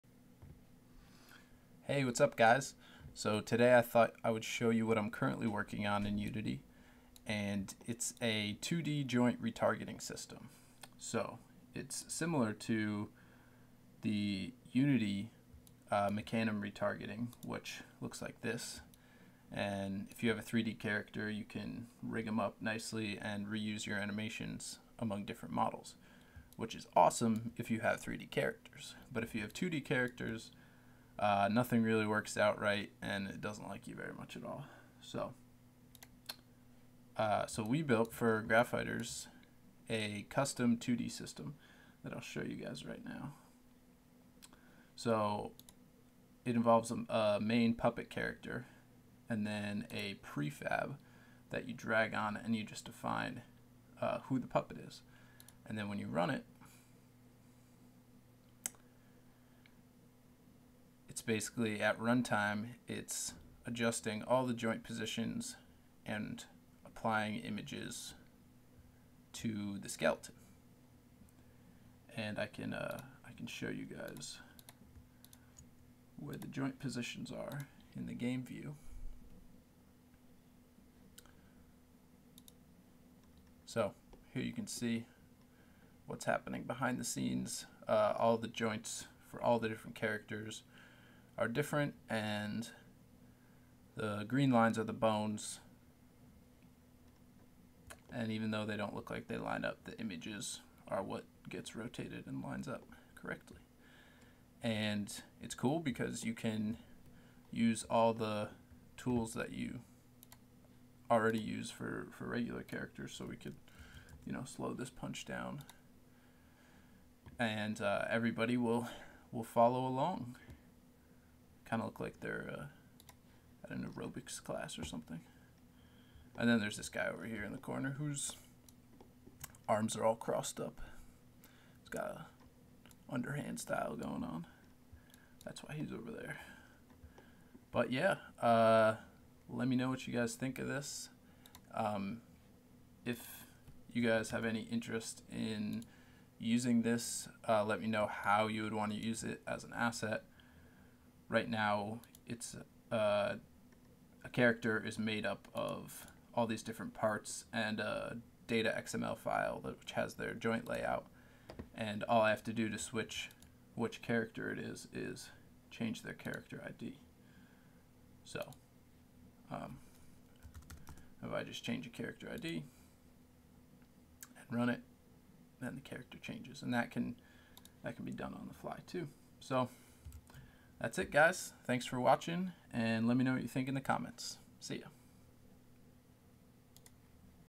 Mecanim 2D Joint Retargeting
Just a little walkthrough of a project im currently working on.